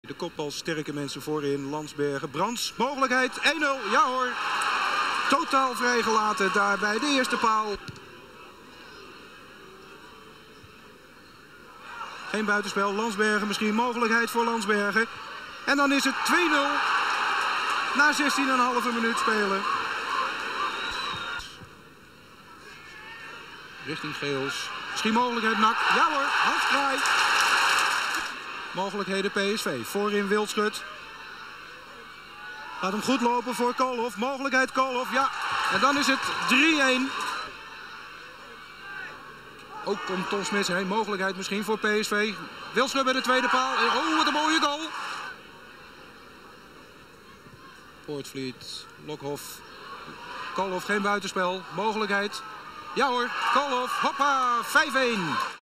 NAC - PSV (19 maart 1983): 1-5
Op 19 maart 1983 speelde PSV in Breda de competitiewedstrijd tegen NAC en won met 5-1. Jurrie Koolhof scoorde twee keer, de overige doelpunten kwamen op naam van Rob Landsbergen, Ernie Brandts en Piet Wildschut.